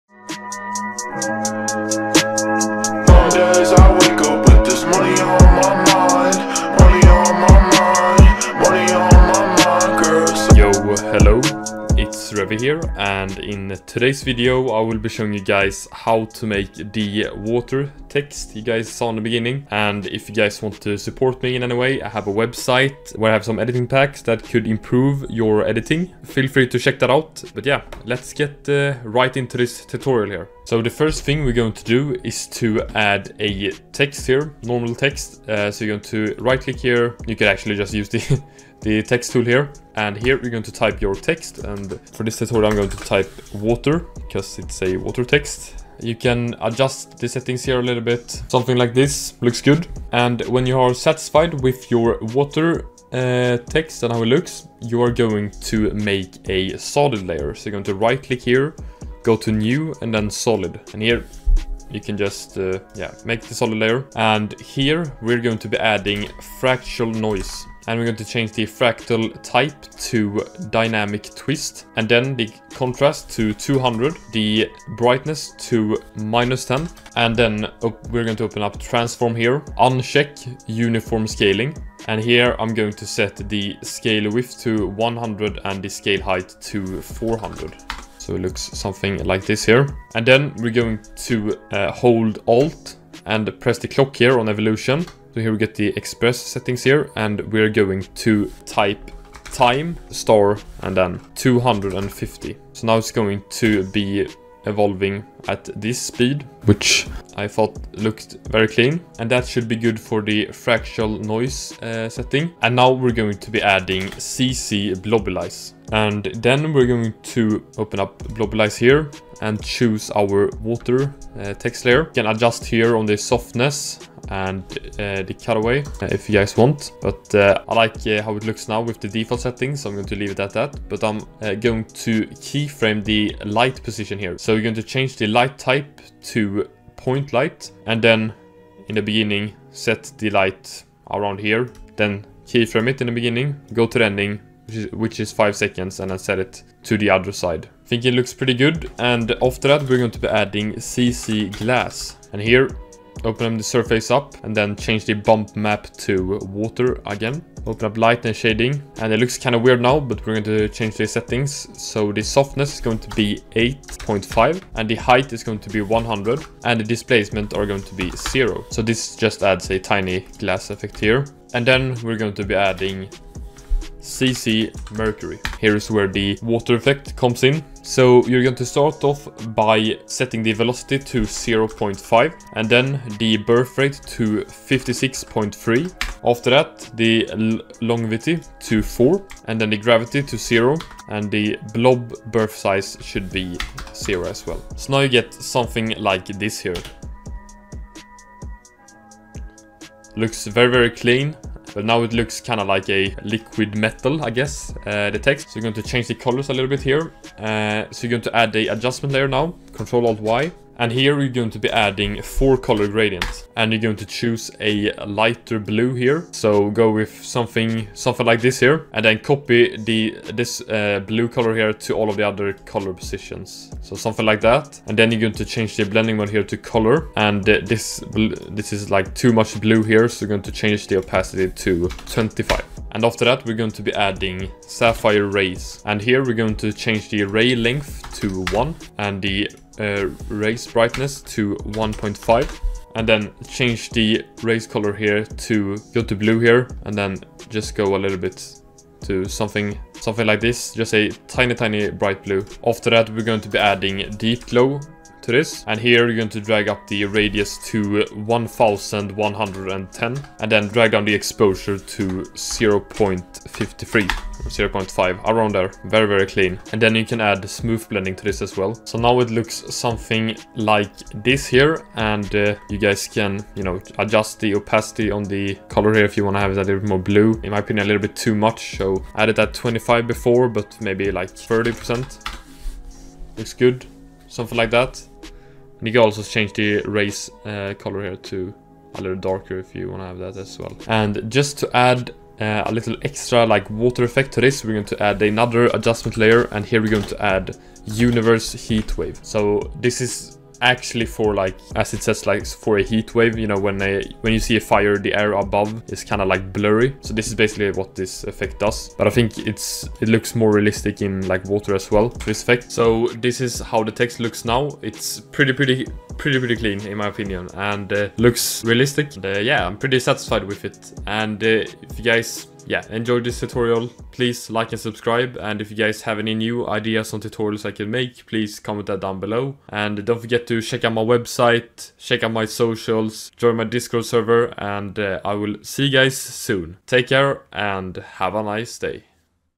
WATER TEXT - After Effects Tutorial
In todays video I'm showing you guys how to make a Water Text in After Effects. Hope you guys enjoyed the video! Love Y'all 💙